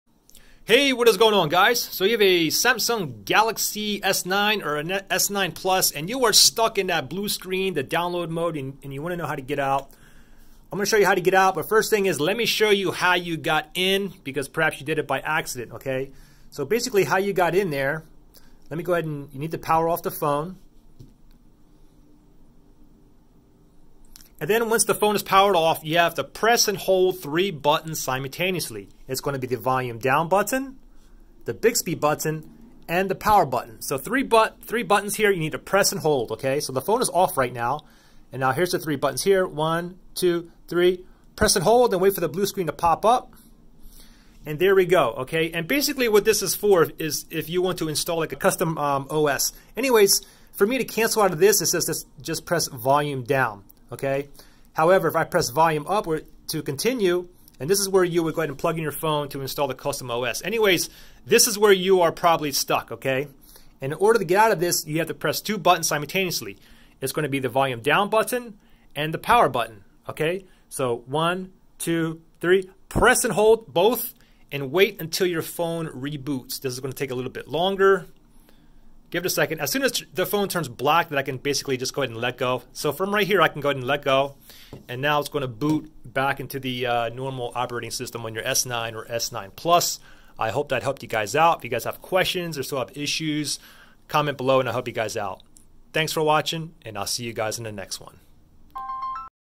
Galaxy S9 / S9 Plus: How to Get Out of "Downloading... Do Not Turn Off Target"
In this tutorial i show you how to exit or get out of the download mode.  You use this feature to install a custom OS, root or flash your Samsung Galaxy S9 or S9 Plus however to get out is simple. Your screen maybe stuck on the blue screen with a down arrow logo with the words, "Downloading... Do Not Turn Off Target. But first let me show you how to get into it if you did this by accident.  Hope this helps thanks.